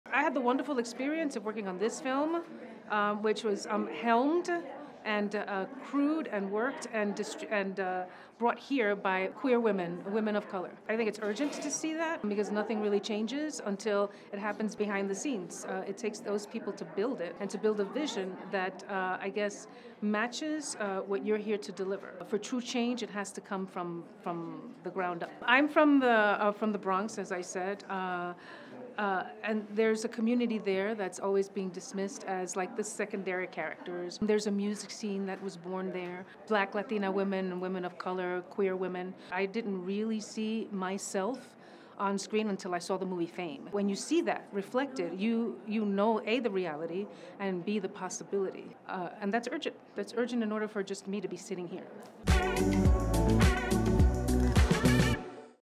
Judy Reyes @ Sundance Film Festival | IndieWire Studio Adobe
It’s urgent that we see, hear, and celebrate diverse perspectives — both behind and in front of the camera. Judy Reyes shares how her career has been impacted by representation in filmmaking. 🎥 @indiewire @sundancefilmfestival